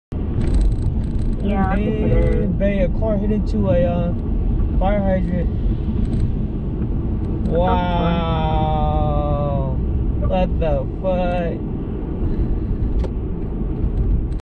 Drive pass a car that hit a fire hydrants
In the AM by 99 and Laguna area.